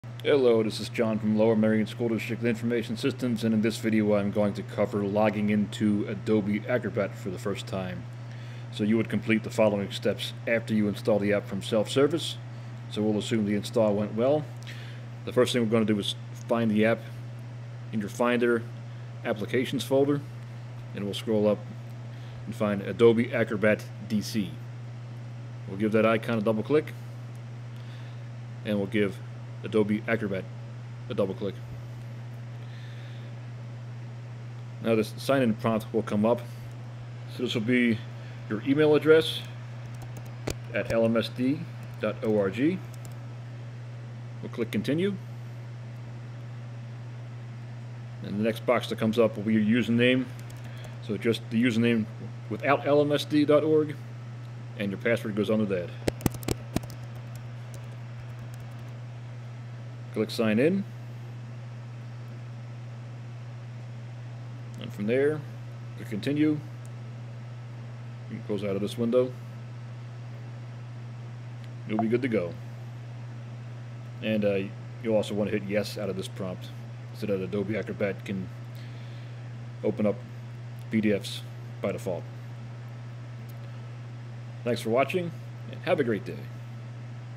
Logging into Adobe Acrobat
The following steps should be completed after installing the Adobe Acrobat DC app via Self Service